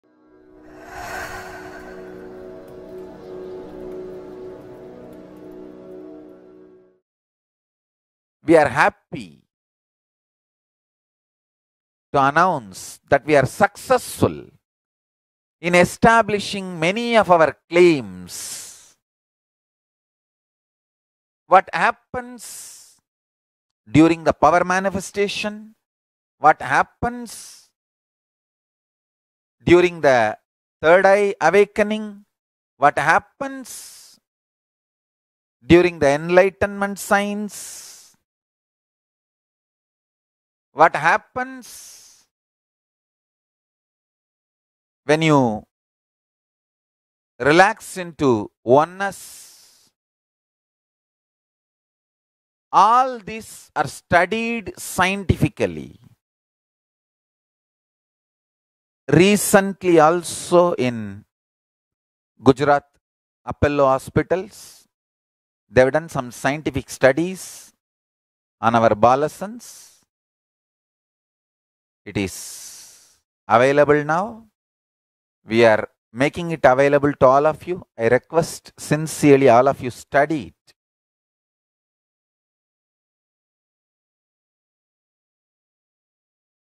Science is Supporting Our Claims
We are happy to announce that we are successful in establishing many of our claims, what happens during the power manifestation, what happens during the third-eye awakening, what happens during the enlightenment science, what happens when you relax into oneness, all these are studied scientifically. Recently also in Gujarat, Appolo hospitals they have done some scientific studies on our Balasanths, it is available now, we are making it available to all of you. I request sincerely all of you study it.